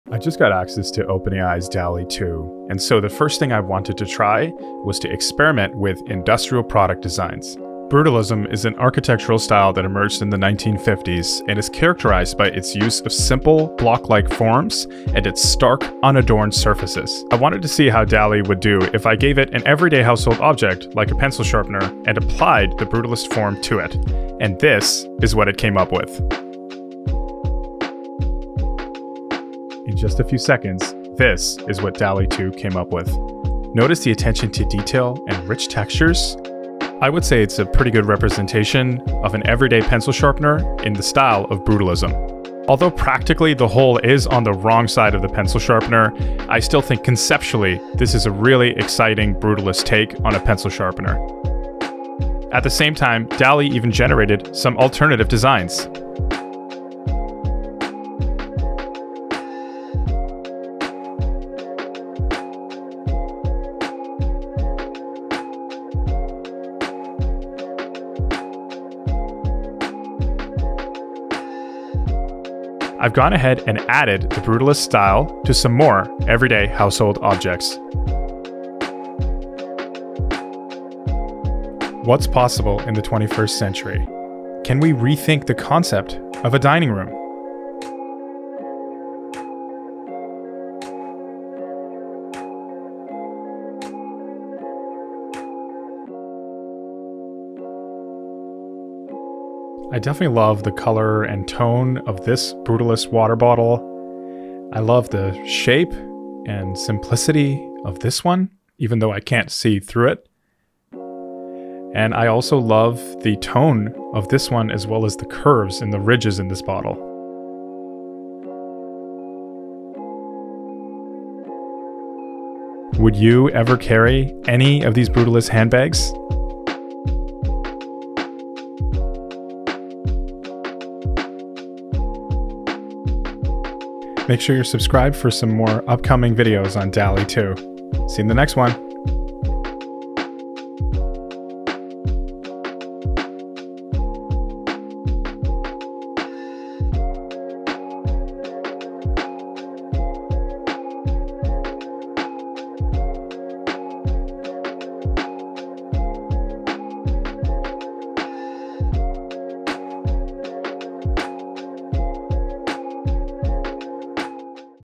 Generating a Brutalist Pencil Sharpener, Water Bottle, and More with DALL-E 2
Generating brutalist industrial product designs for various household objects using DALL-E 2

btw - I'm aware a purse is technically not an industrial design object, more like fashion design, but I left it in there anyways :)